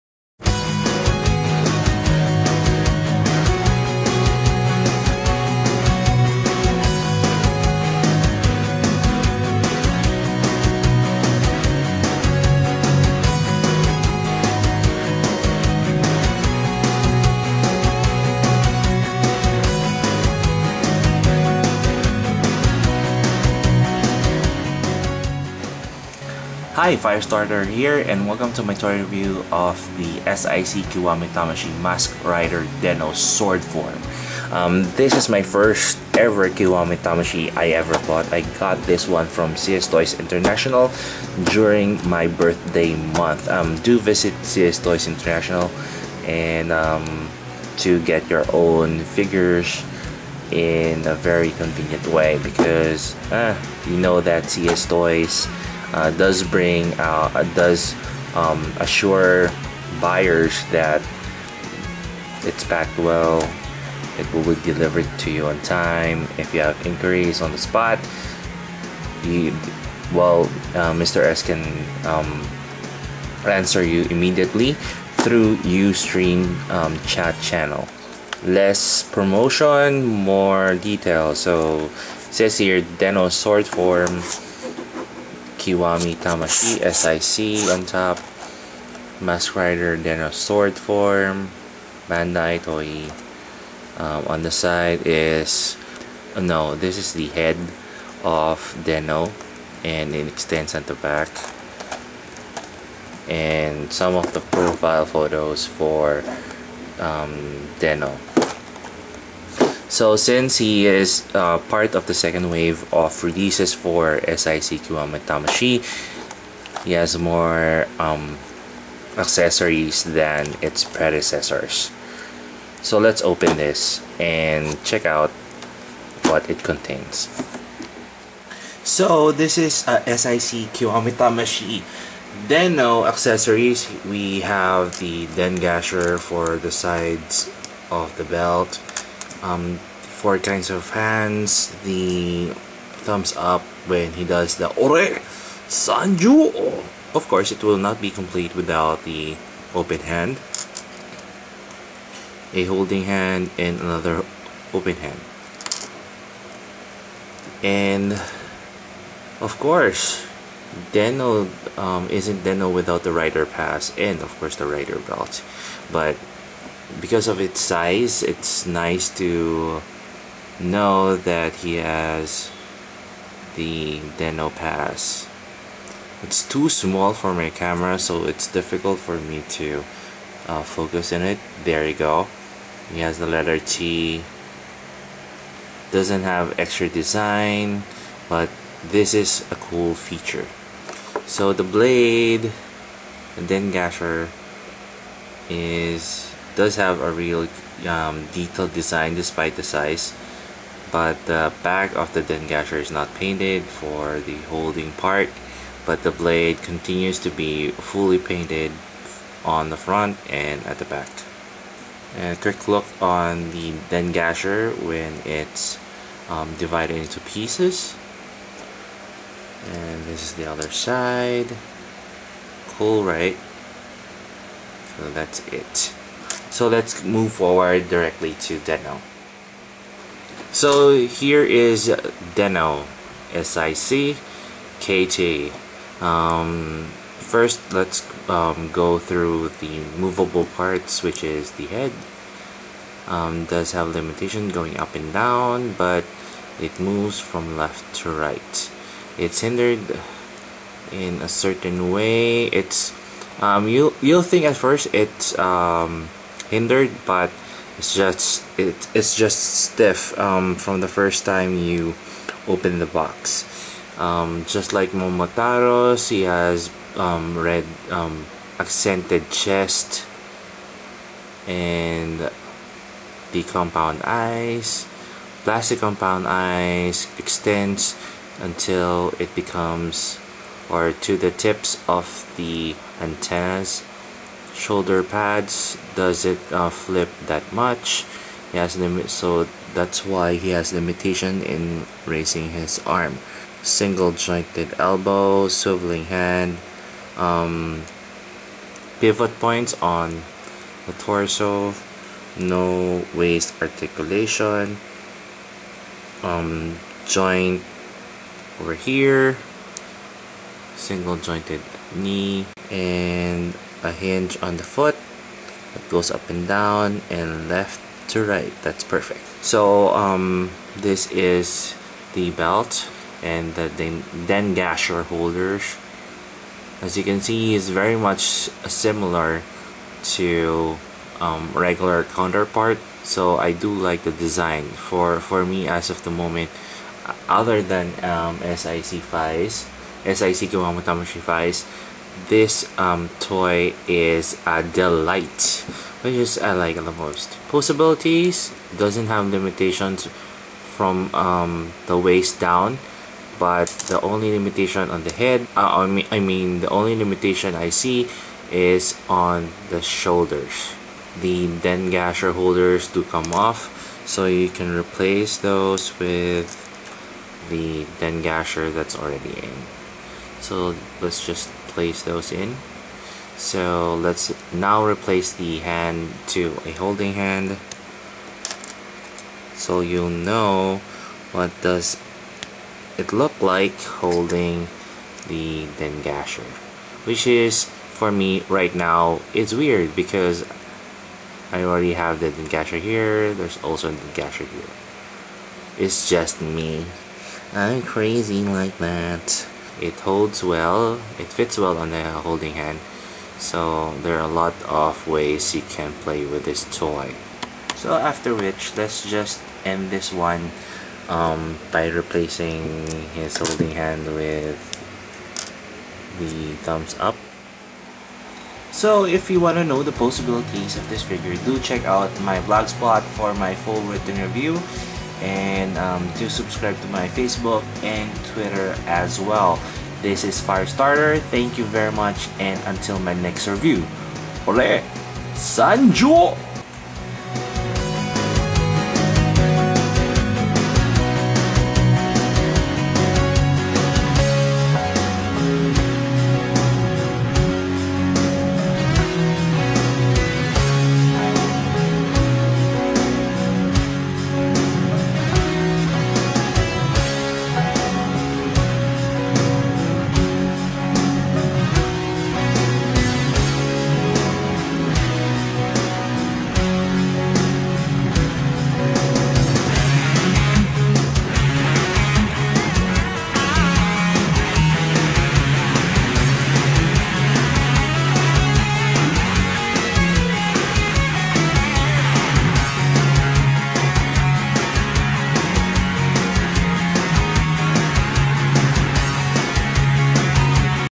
Toy Review: SIC Kiwami Tamashii Den-O Sword Form
This is my first purchase from CStoys International for my birthday month. So far, this is the best looking SIC KT I've ever seen yet. Gives you more what you bargained for.